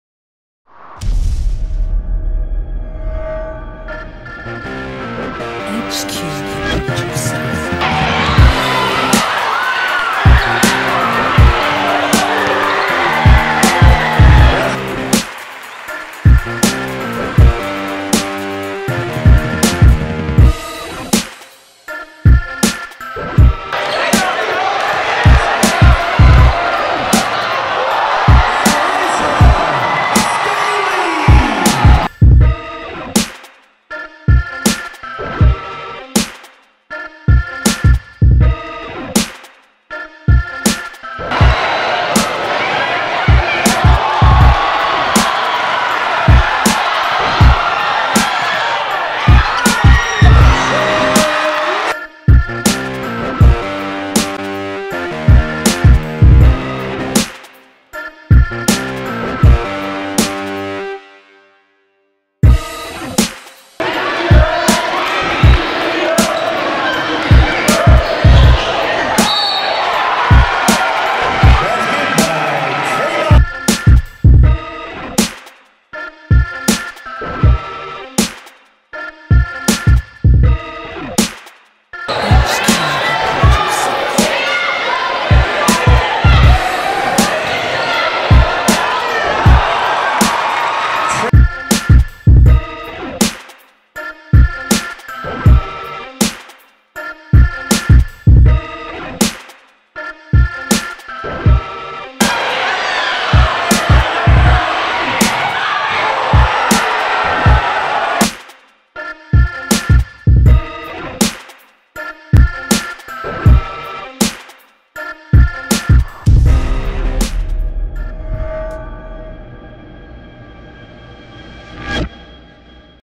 Top 5 Plays of Wheeler vs. Shiloh (12/16/11)
7'0" sophomore Trayvon Reed of Shiloh owned this highlight session with some monster dunks.